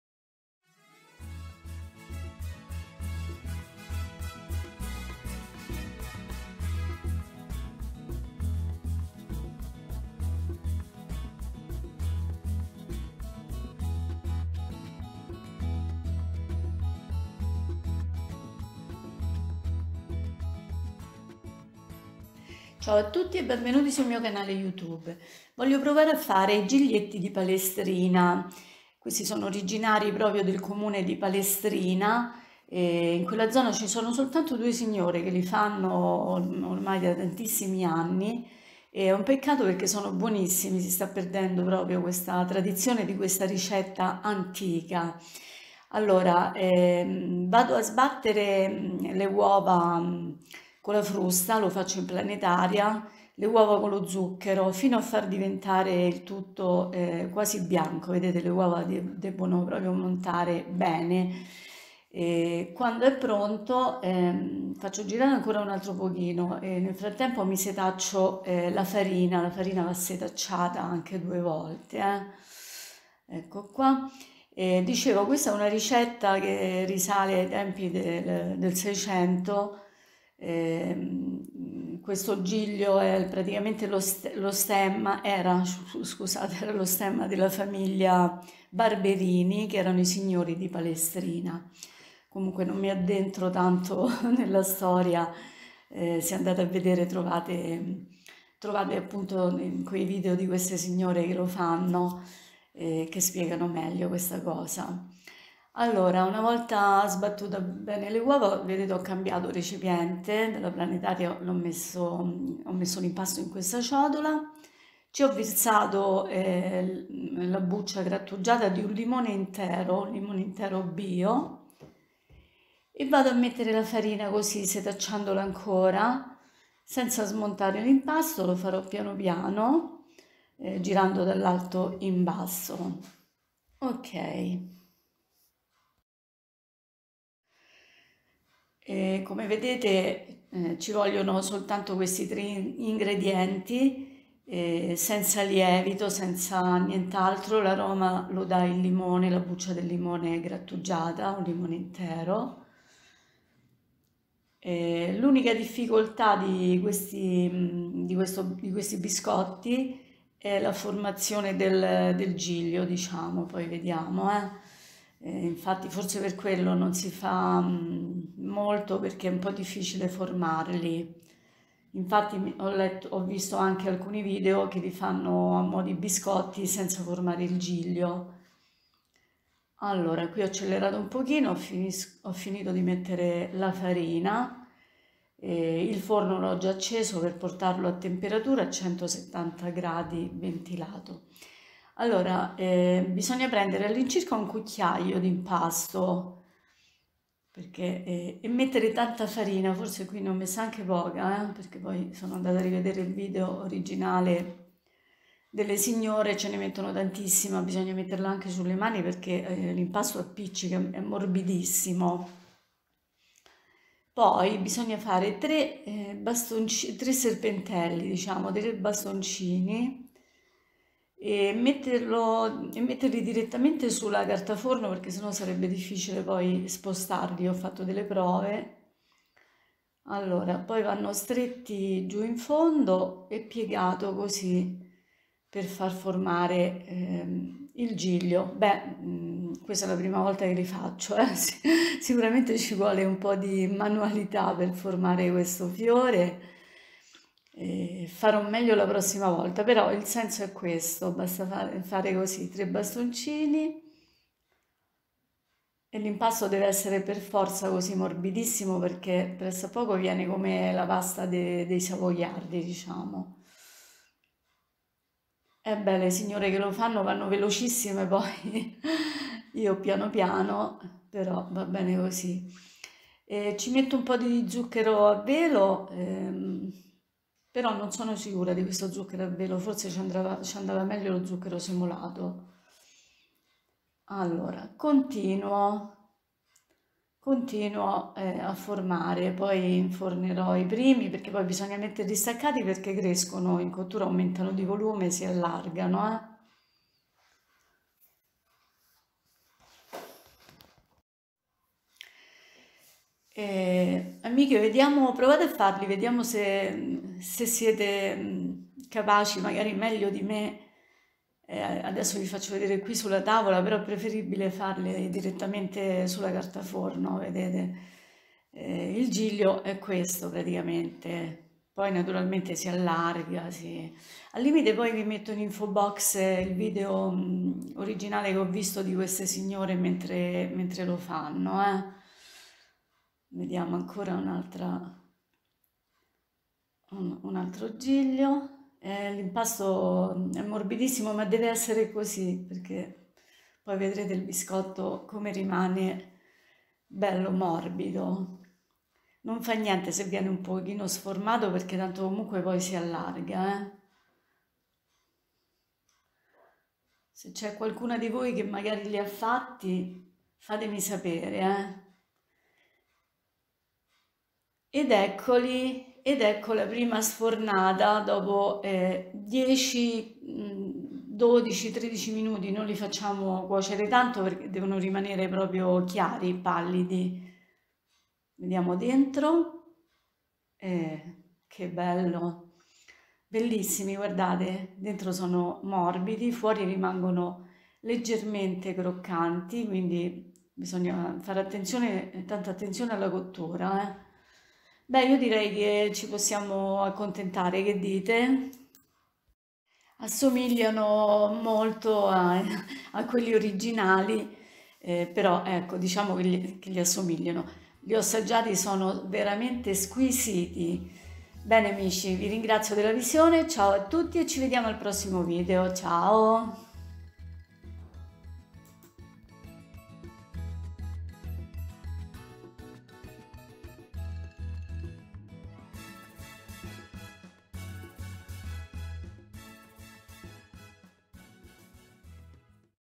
Giglietti di Palestrina
giglietti di Palestrina (Rm) biscotto tipico ricetta storica
con solo uova farina e zucchero aromatizzato con scorza di limone